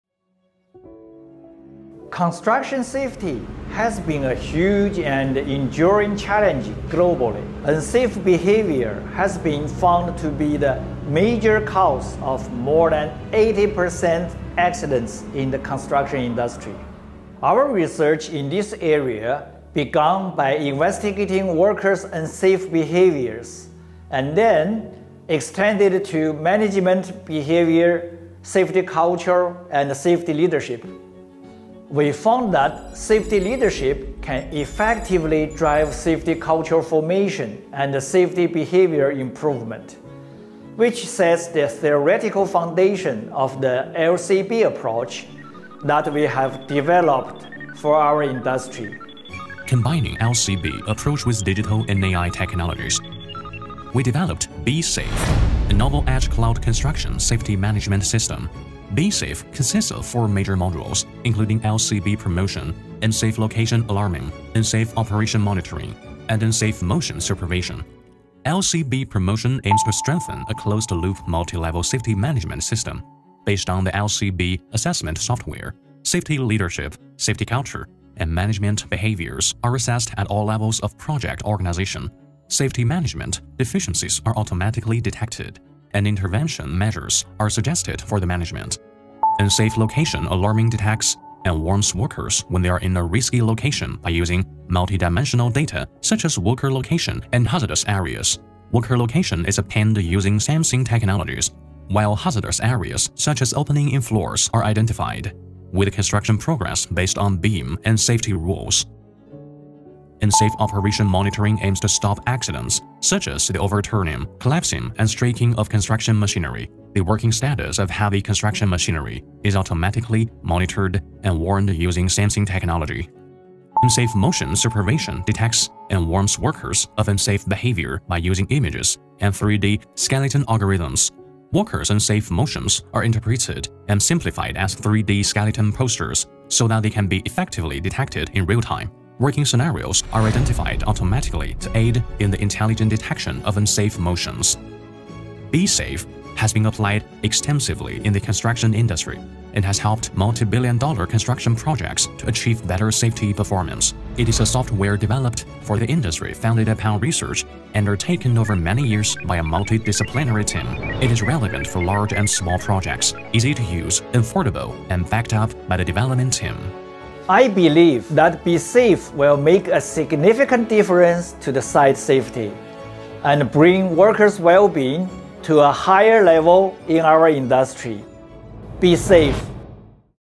International Grand Prize of the CIC Construction Innovation Award 2022
"INTERNATIONAL GRAND PRIZE
國際組 - 創新大奬"

Title Entry 作品名稱：
"Besafe (AI Safety Management System)
Besafe (智能安全管理系統)"

Companies 公司 / Organisations 機構：
"Tsinghua University
Gammon Construction Limited,
Huarui Artificial Intelligence Technology (Beijing) CO. LTD
清華大學
金門建築有限公司
華瑞智源科技(江蘇)有限公司"